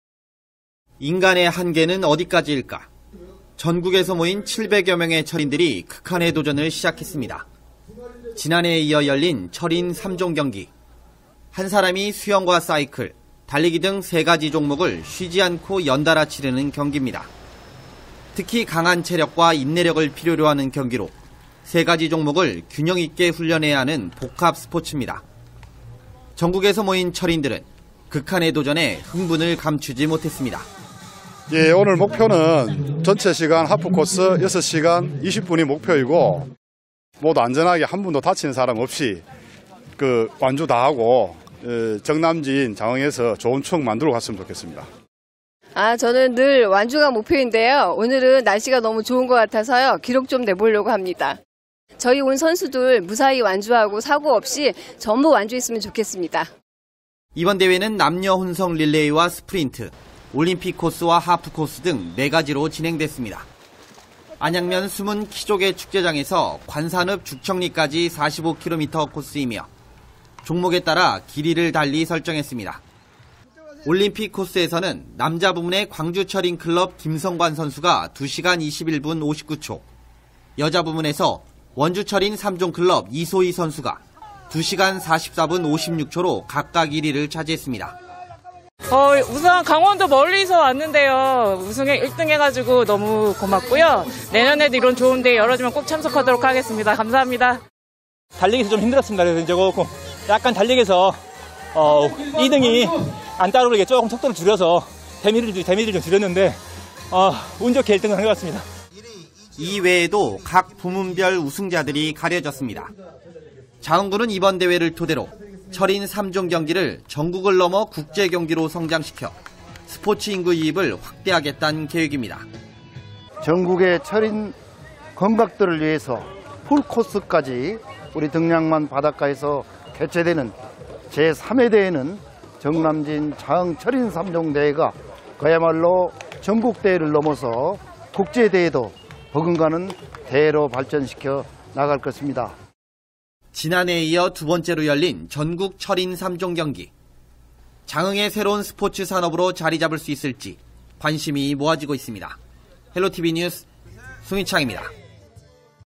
인간 한계 도전 ′철인 3종 경기′
인간의 한계에 도전하는 스포츠, 바로 철인 3종 경기인데요.
한 사람이 달리기와 수영,  사이클 세 종목을 쉬지 않고 연달아 치르는 경기입니다.
전남 장흥에서 전국 규모의 철인 3종 경기가 열렸습니다.